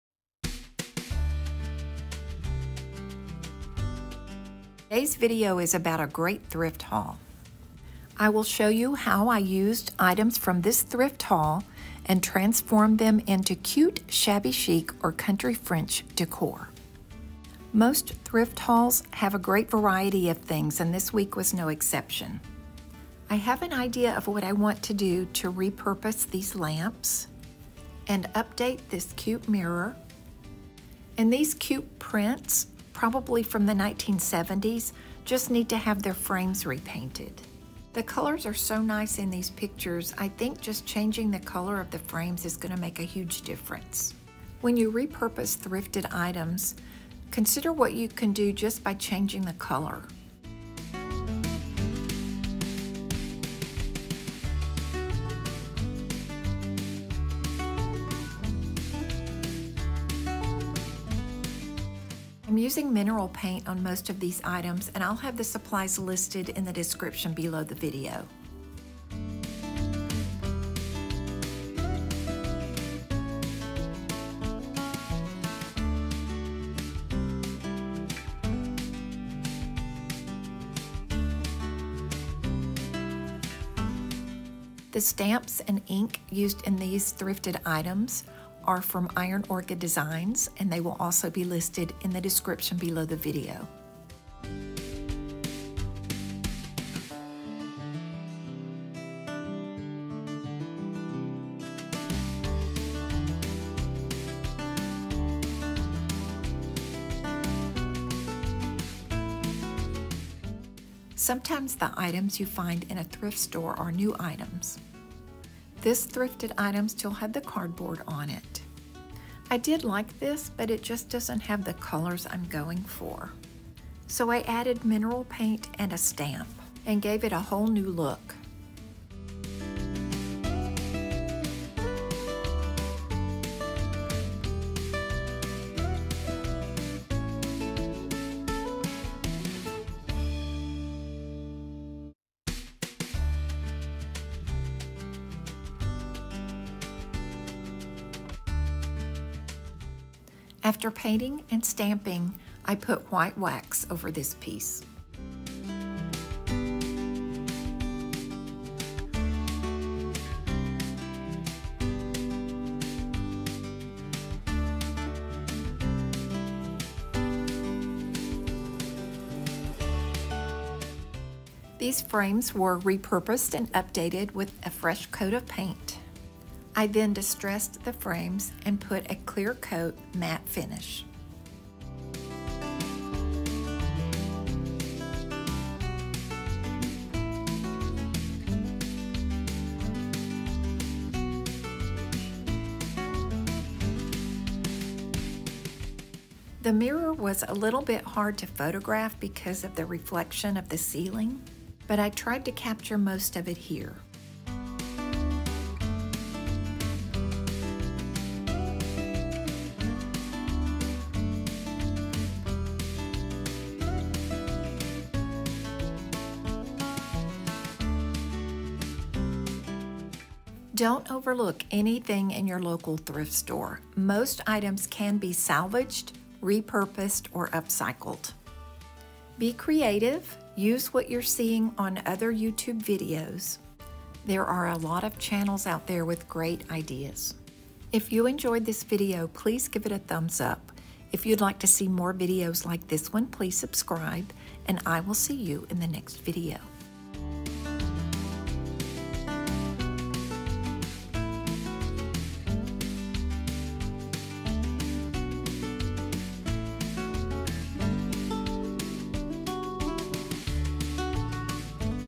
How to Flip Old Tin Decor Items and Art
Look what I found at my favorite thrift store and watch how I repurposed and updated this great thrift haul! DIY thrifted items for beautiful Country French, Shabby Chic, or Farmhouse decor!
Click on the video popup at the end of the video and watch one of most popular videos on decor and flipping thrifted items!

Supplies used in this video:
(I may receive a small percentage on qualifying Amazon purchases at no additional cost to you.)